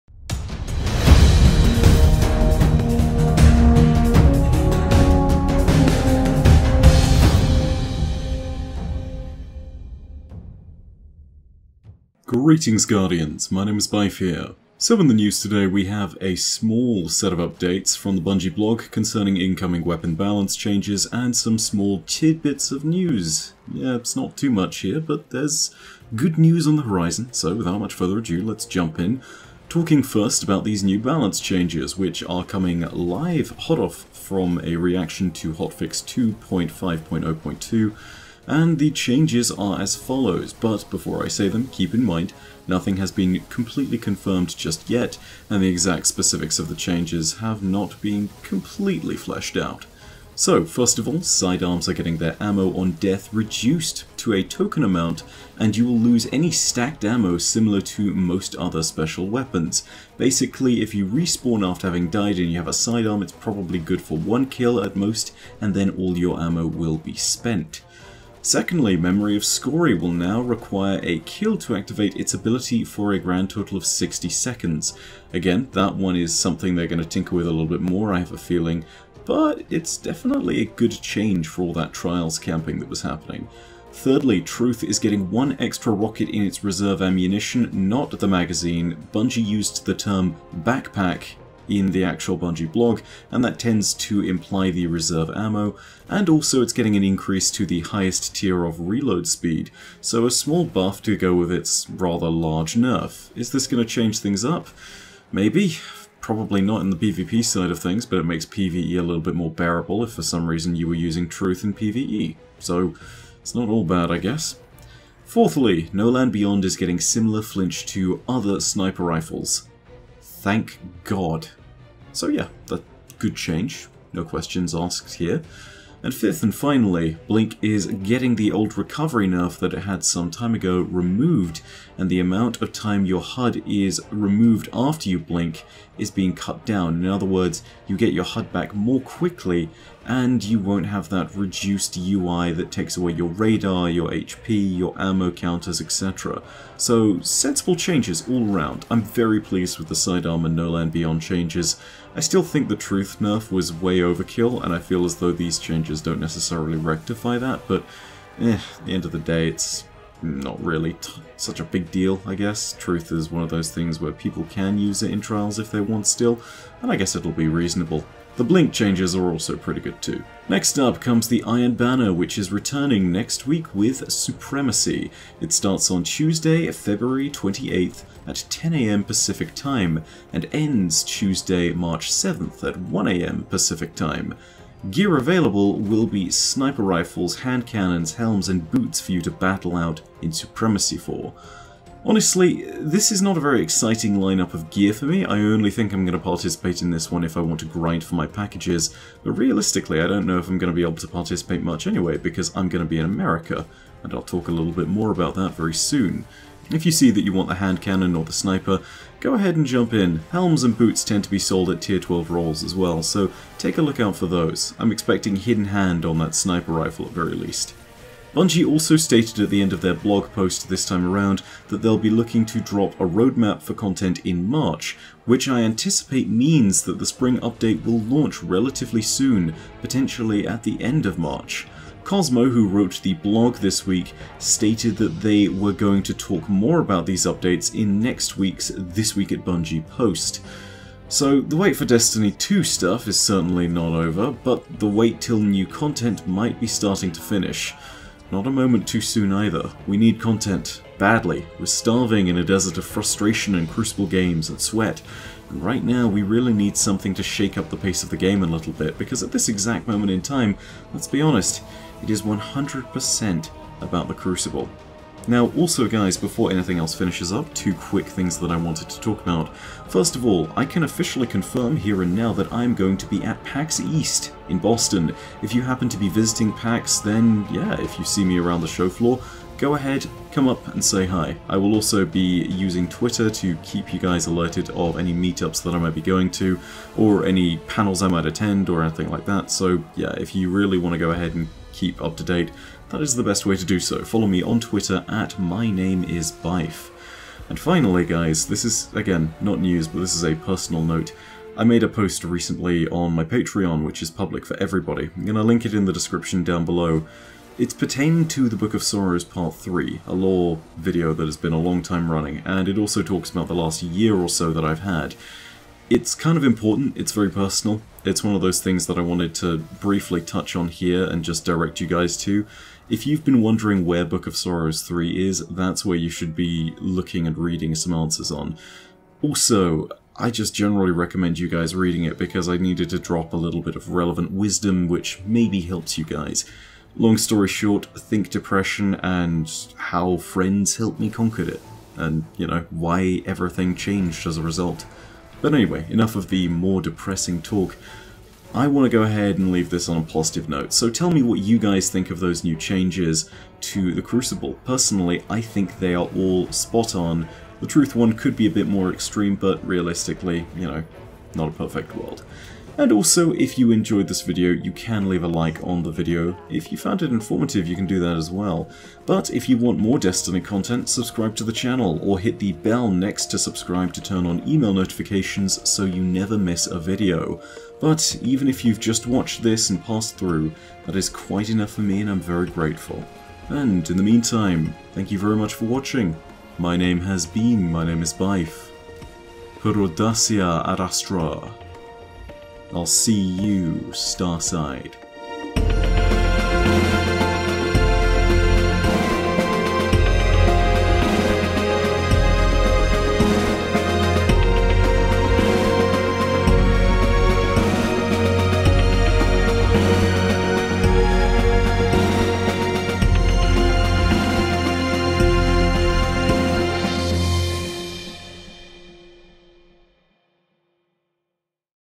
Destiny News - March Content Release? Sidearm, Skorri & NLB Nerf, Iron Banner
Destiny News! Bungie is stating that soon we'll recieve a roadmap for content releases in March. The Bungie Weekly Update also had details of new weapon balance changes affecting No Land Beyond (NLB), Sidearms, Skorri's, Truth and Blink! Iron Banner Supremacy is also returning.